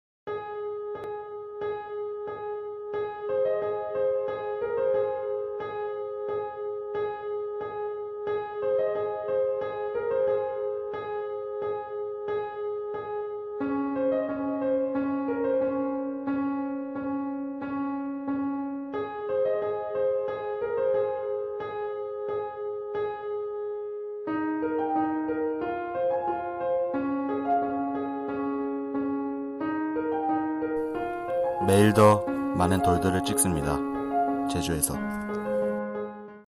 Photos Everyday In Jeju Stone ㅣ 제7회 제주10대문화 UCC공모전 18
제주 삼다 중에는 돌이 들어갈 정도로 제주에는 돌이 정말 많다. 돌이 많아서 제주에는 타지에 비해 돌 문화가 많이 발전 해 있다. 그래서 우리 팀은 주제로 가장 흔하면서도 아름다운 모습을 담고 있는 제주 돌 문화를 선택하였다. 돌담, 돌하르방, 불턱 등 제주의 돌 문화와 제주의 아름다운 환경을 조화 하여, 대중들이 많이 아는 아이폰 광고로 패러디 하였다. 중간 중간에 돌뿐만이 아니라 제주의 해녀와 제주 초가도 넣어 돌뿐만이 아니라 제주의 다른 모습도 보여주었다. 30초란 짧은 시간 안에 표현하기란 쉽지 않았지만  사람들에게 제주 돌들과 제주의 자연환경을 알려 주기 위하여 직접 돌아다니며 제주의 돌들을 찍었다. 어떠한 특별한 메시지나 의도를 담은 것이 아닌 제주의 돌들의 모습을 감성을 담아 영상으로 만들었다.

원작 : 아이폰 광고